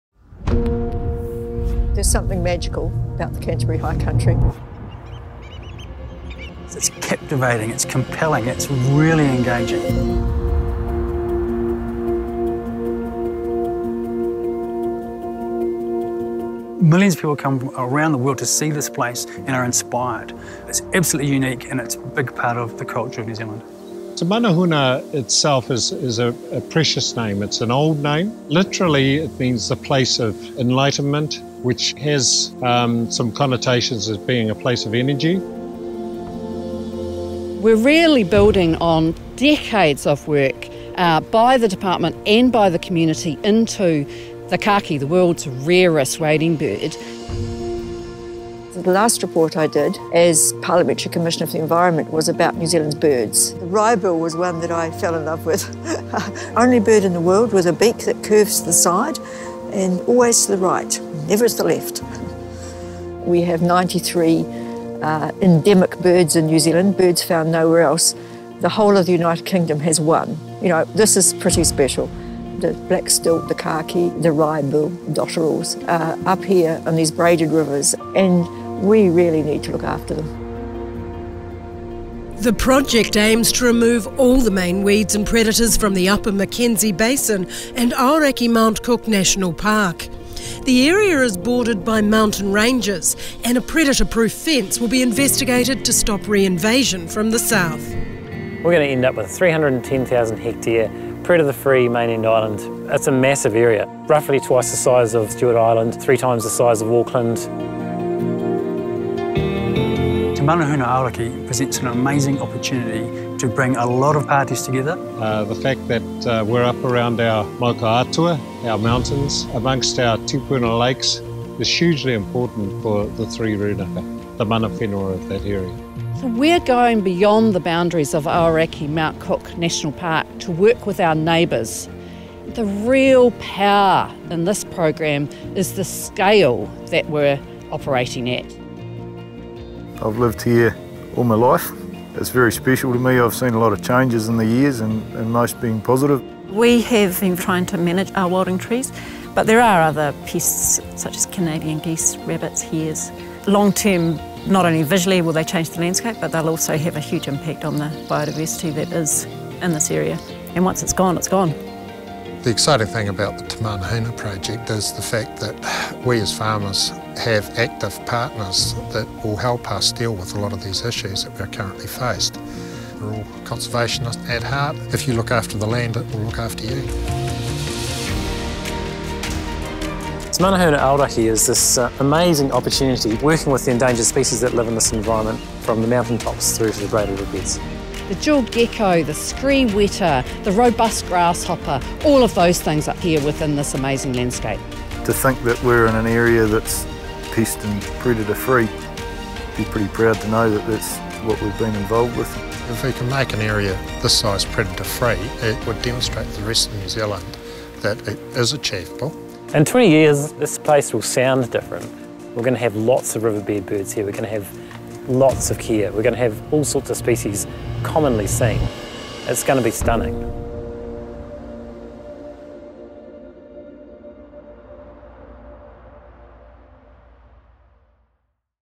Te Manahuna Aoraki project
Te Manahuna Aoraki is biodiversity project that will create a vast 310,000 hectare predator free mainland island in the Upper Mackenzie basin and Aoraki / Mt Cook National Park.
The project will preserve these unique landscapes, securing a safe habitat for endangered species ranging from kea and rock wren in the alpine zone to braided river species like wrybill, robust grasshoppers, jewelled gecko and the world’s rarest wading bird - the kakī, black stilt.